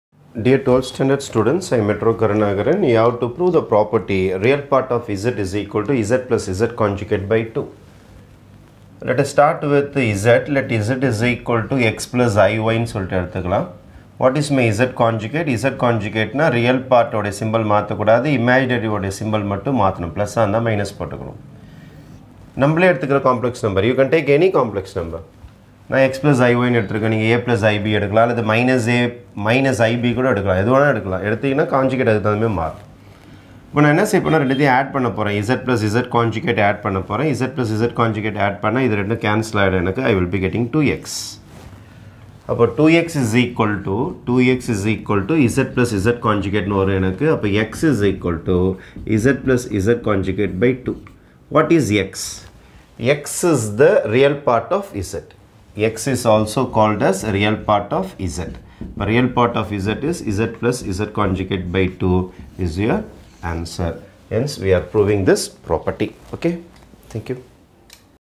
12th Std Maths Chap 2: Property 5 Prove that Re(z) = (z + conjugate of z)/2
12th Std MATHEMATICS